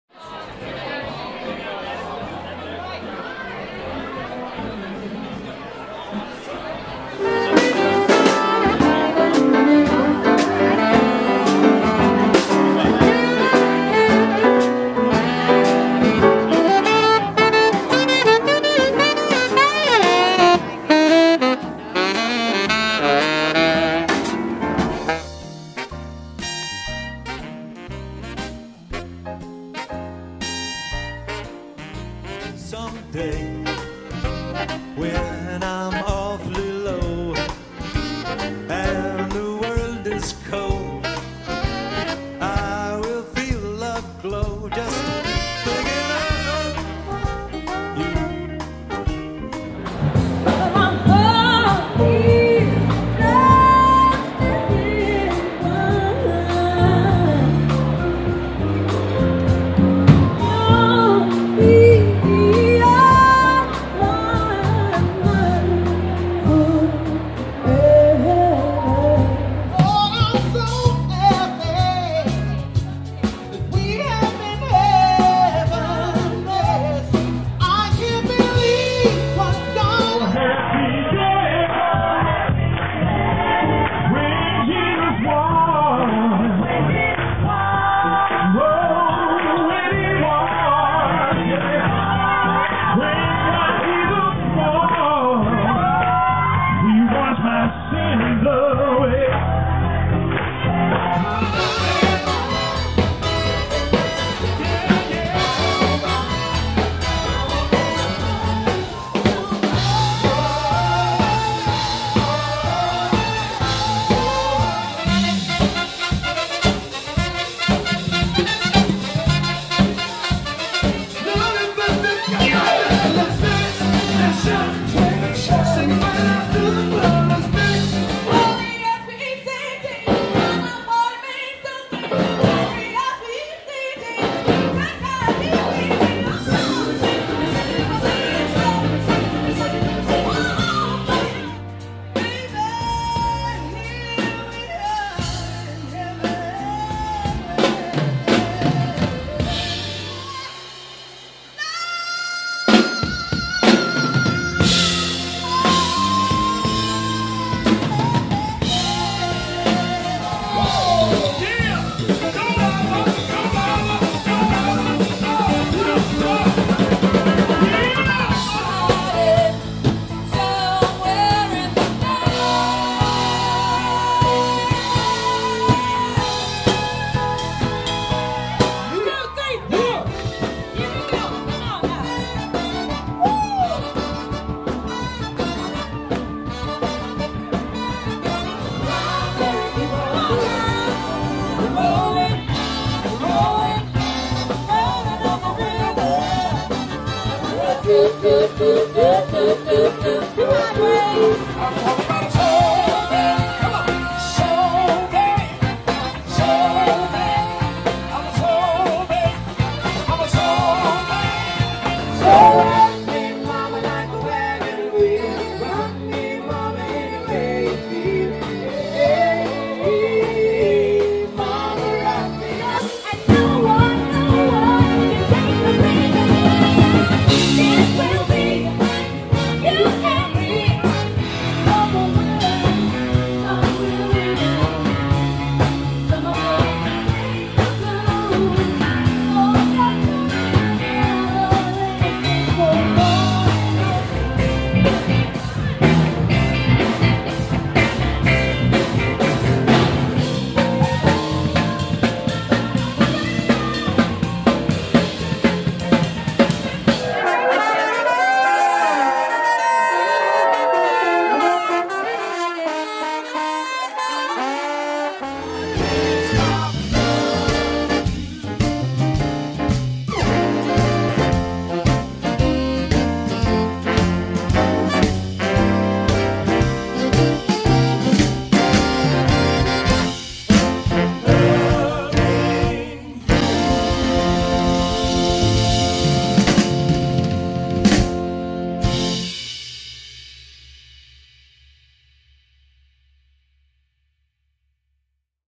Wedding Sampler 2013
Live samlle vids from recent Pat Patrick Band wedding performances